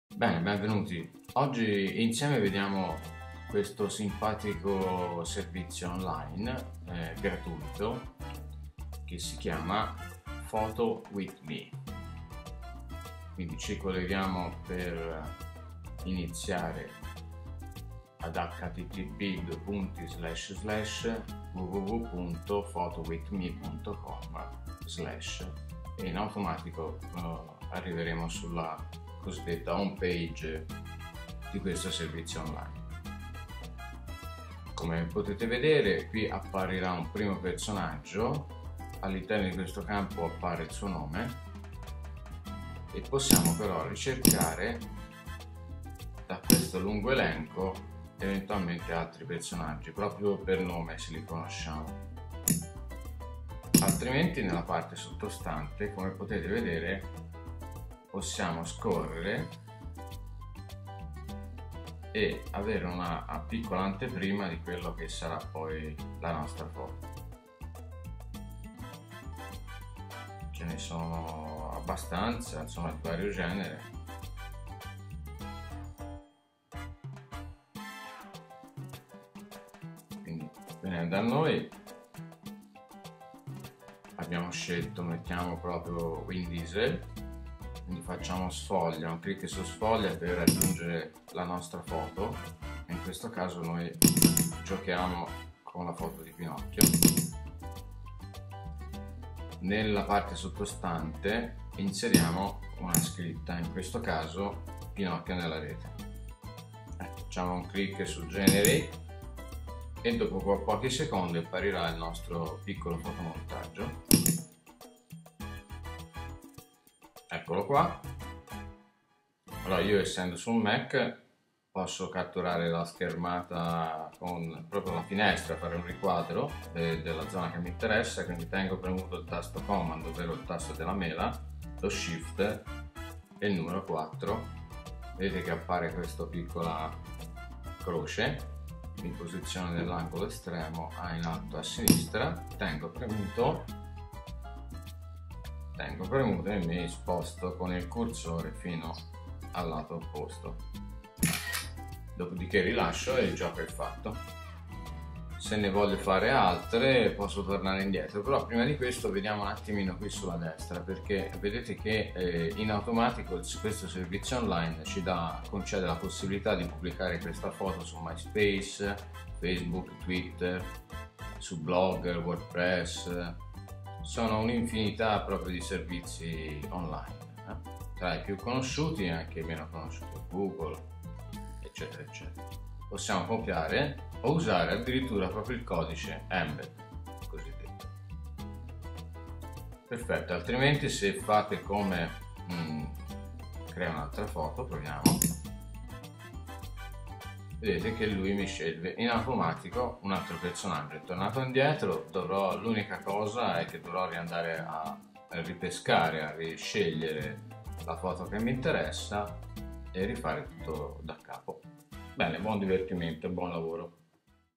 PhotoWithMe
Scopriamo insieme come utilizzare PhotoWithMe... questo simpatico servizion online, gratuito, che ci permette di poter realizzare dei simpatici fotomontaggi con dei personaggi di un certo rilievo...